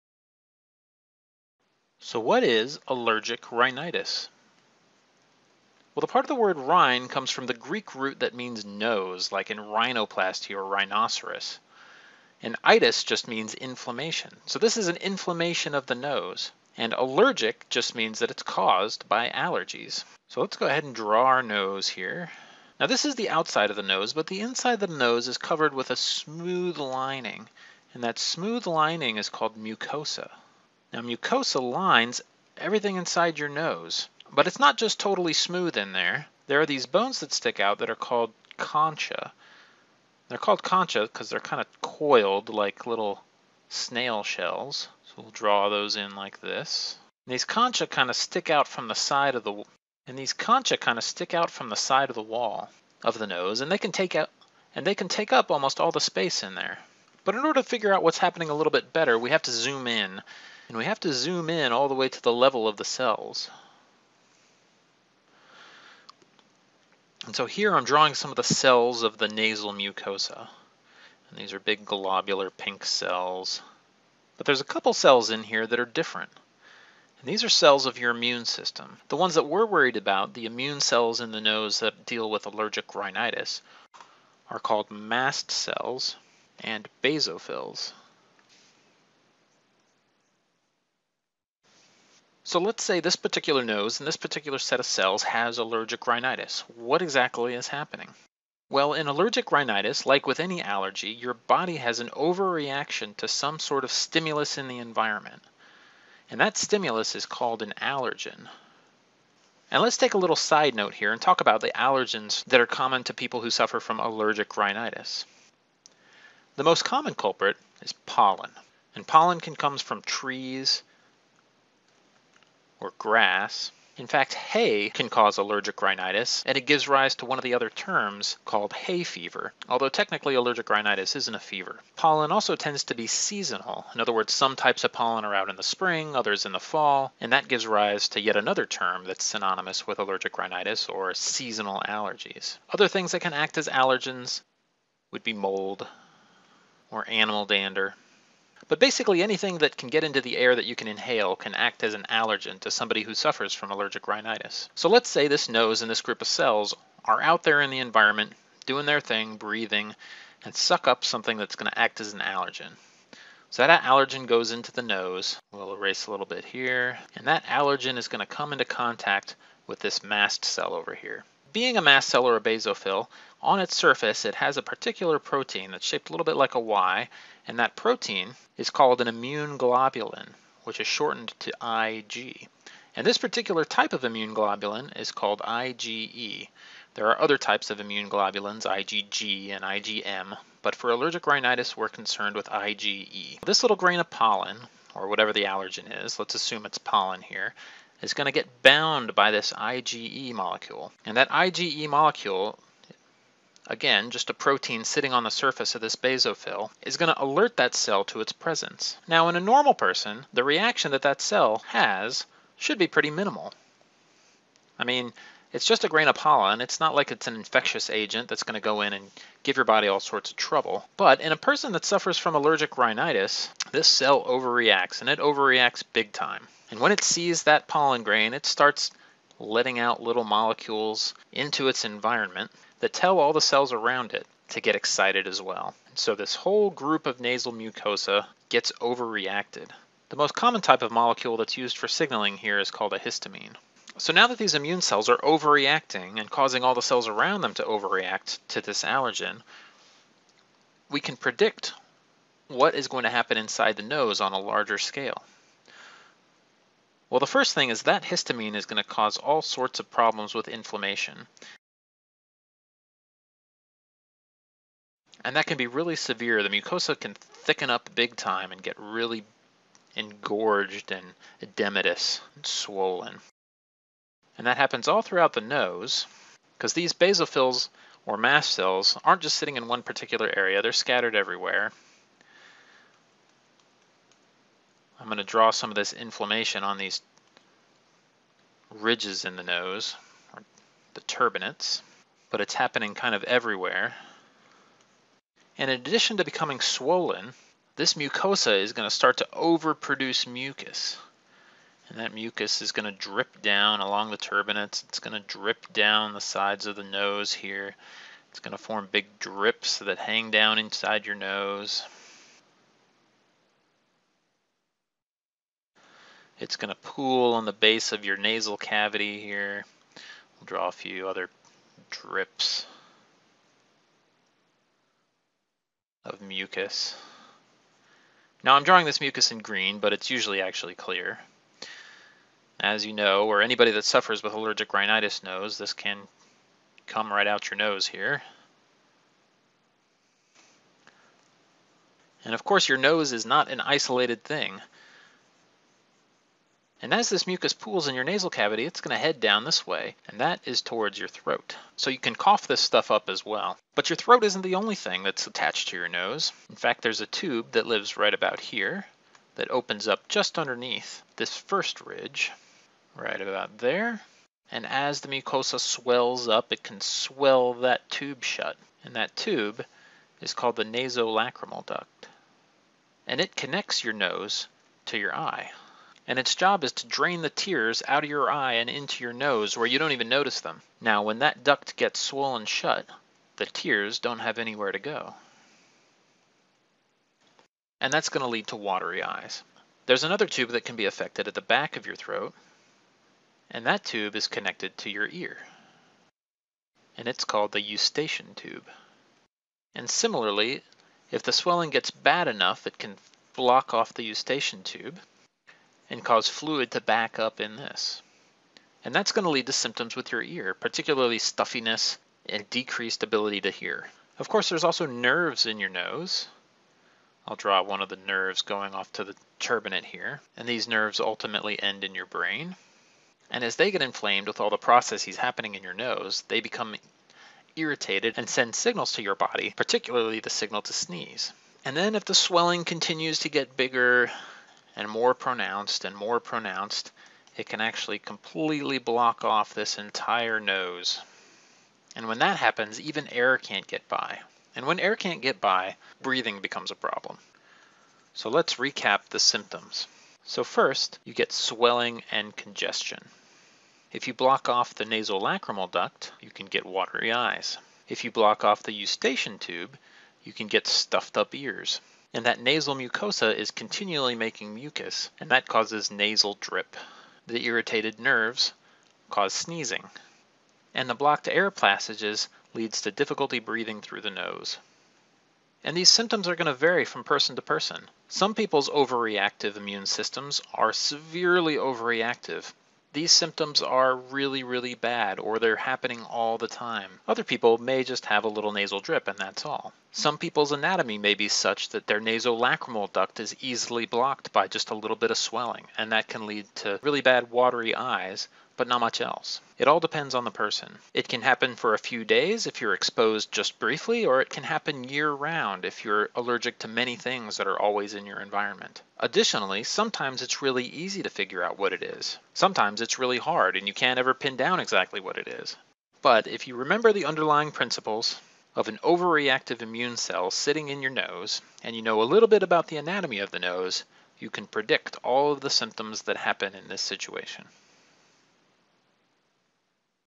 What is allergic rhinitis? | Respiratory system diseases | NCLEX-RN | Khan Academy
Created by Jeff Otjen.